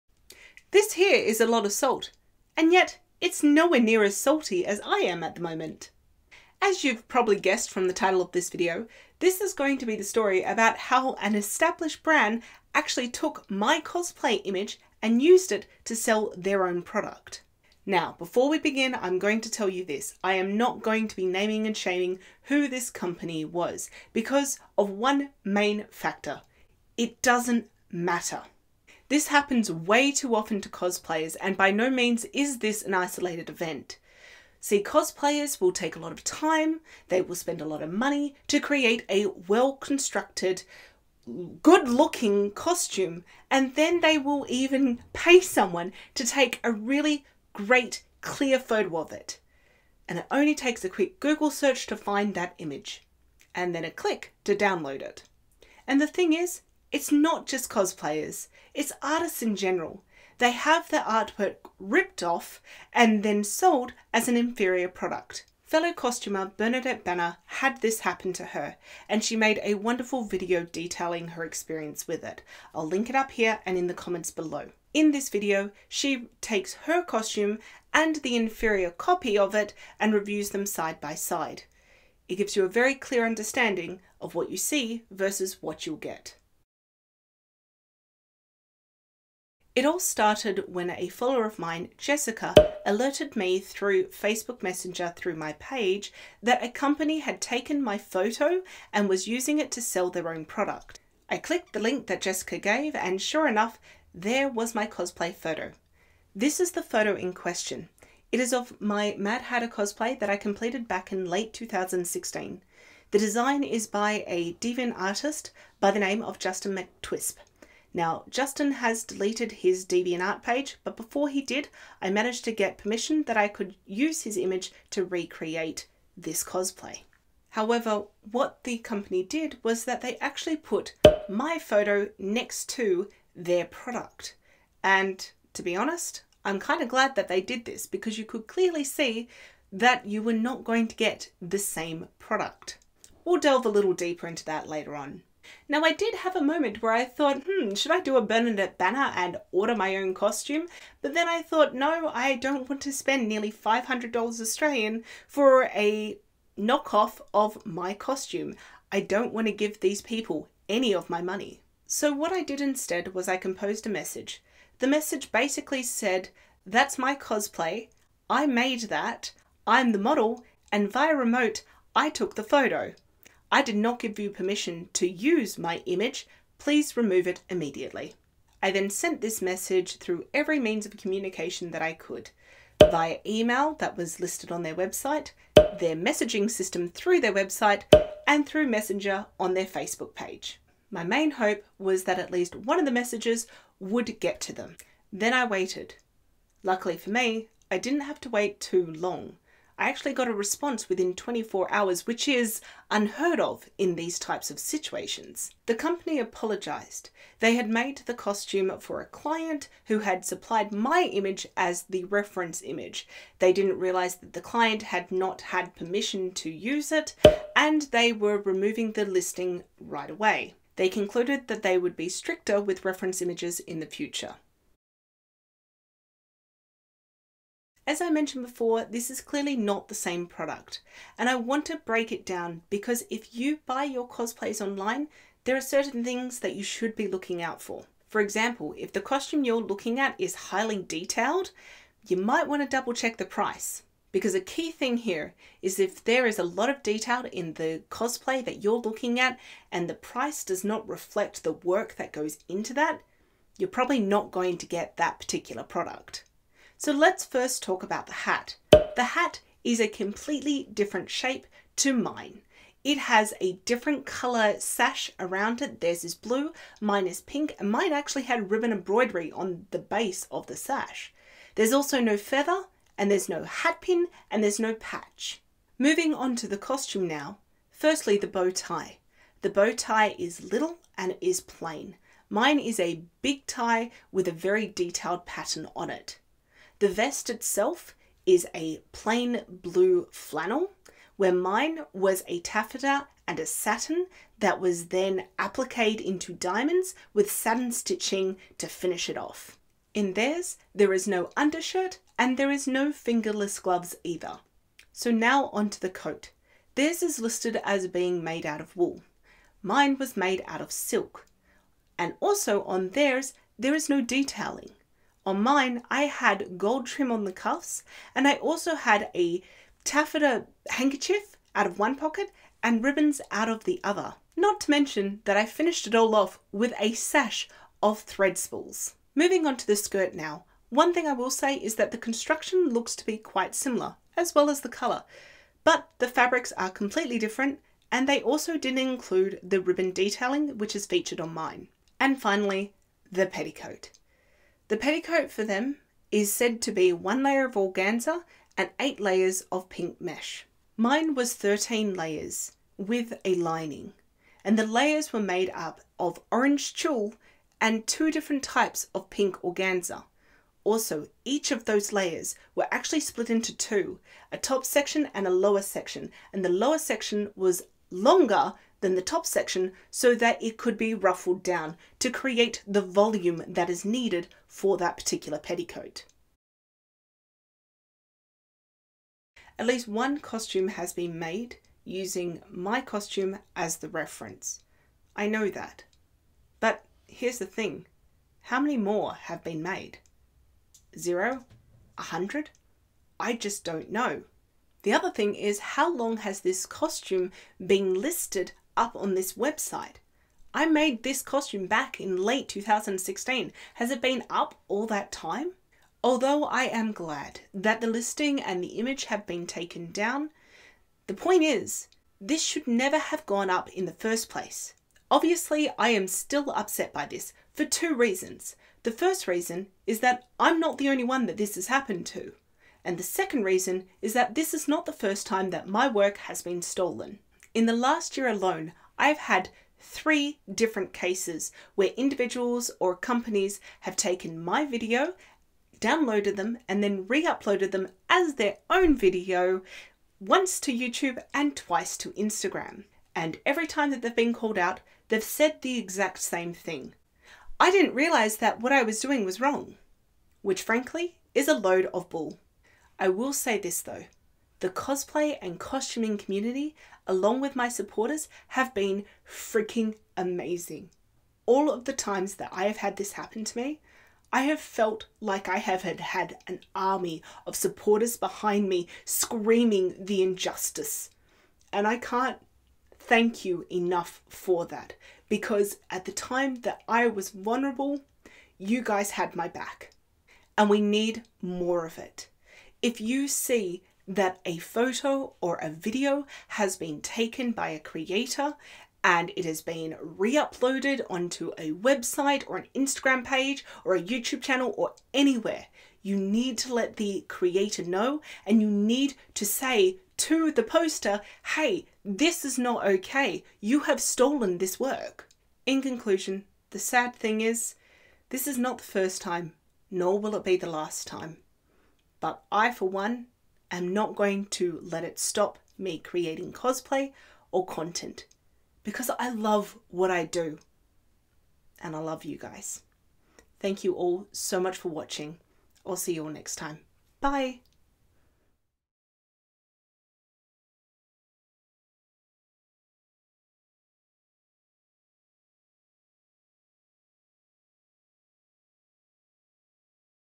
KNOCKOFF ALERT! A company STOLE my cosplay image to sell their own product!
Recently, an establish brand took one of my images and uploaded it to help sell their own product. I did not give them permission to do so.
In this case, I am one of the lucky ones - after I called them out on it, they removed the image and listing immediately, but it should never have happened in the first place. And if I had not raised this with them, it would still be up.
This is not okay.

And the real sad part? I am not alone in this. This happens all the time to artists - whether it be cosplays, costumers, visual artist, stock artists, content creators - their work is stolen, and then re-uploaded or sold as an inferior product. No credit given.

In this video I talk a little about my experience with this situation, but I've also had my videos stolen before and when I was a stock artist, I had those images stole.

Time stamps:
1:27 - What Happened
3:46 - Comparing the Cosplays
6:19 - Final Thoughts